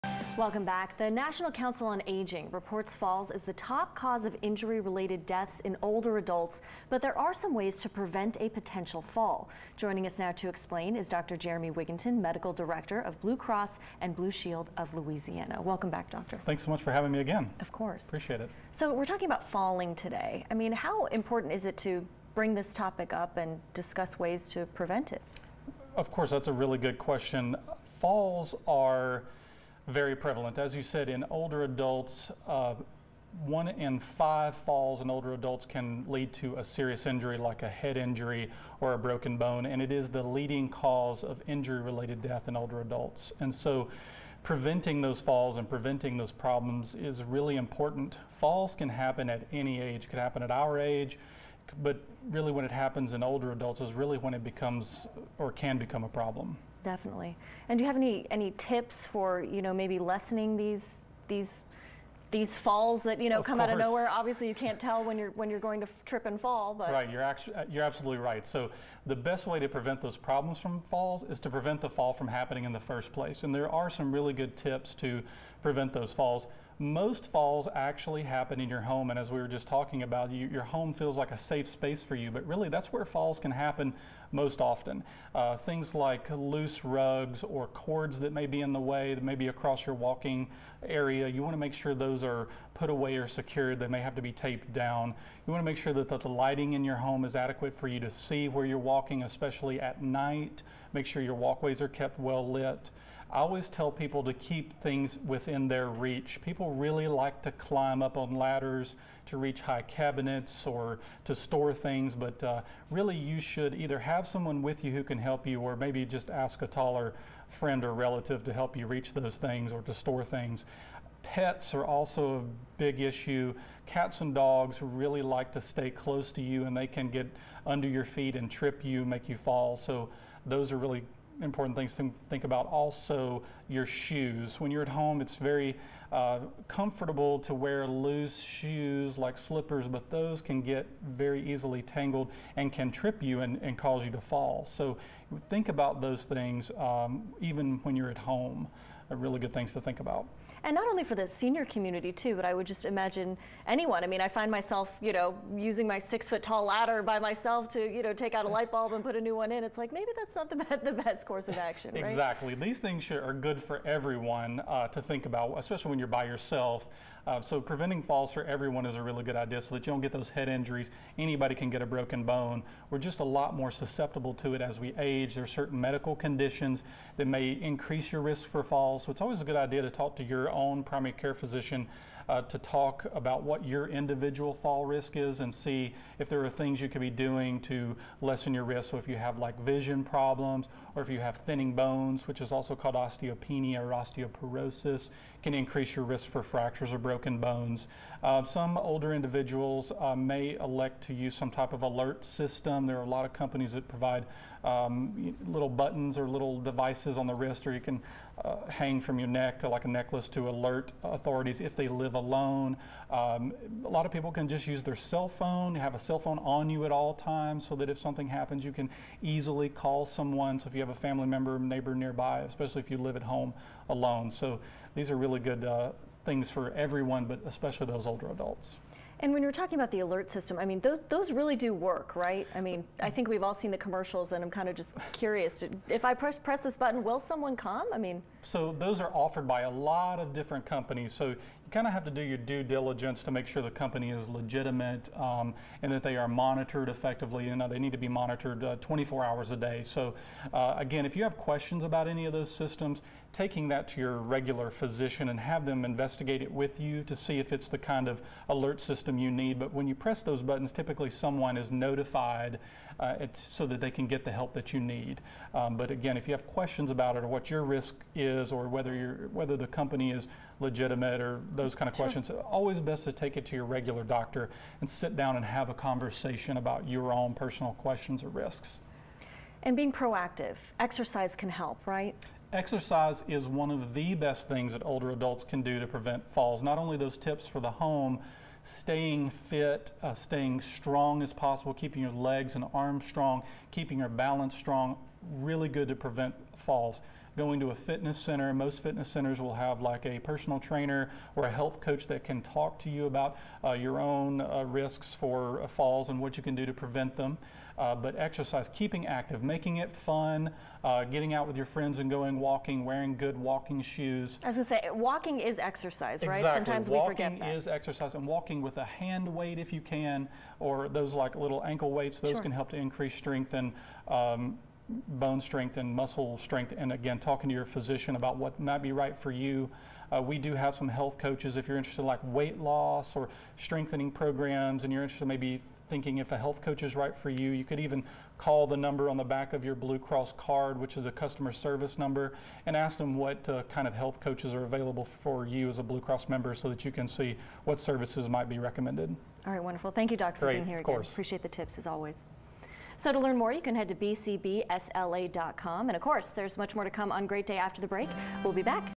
Blue Cross and Blue Shield of Louisiana
Safety Tips for Seniors! Blue Cross Medical Director Dr. Jeremy Wigginton joins us to talk about fall prevention facts, simple and easy tips to reduce tripping hazards in your home, and why you should talk to your doctor about fall prevention. For more go to BCBSLA.com.